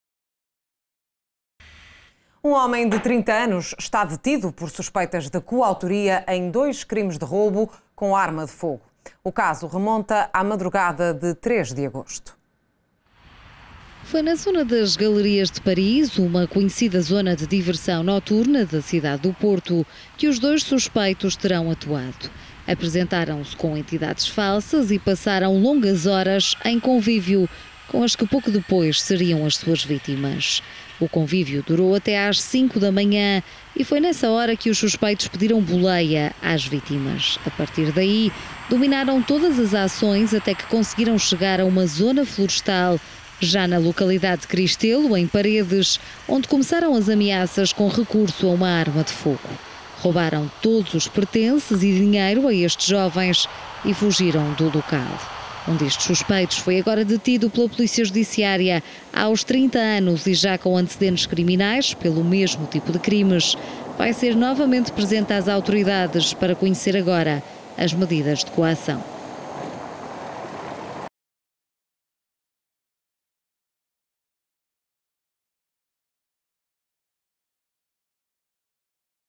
Homem detido em Paredes
Um homem de 30 anos está detido por suspeitas de coautoria em dois crimes de roubo com arma de fogo. O caso remonta à madrugada de 3 de agosto.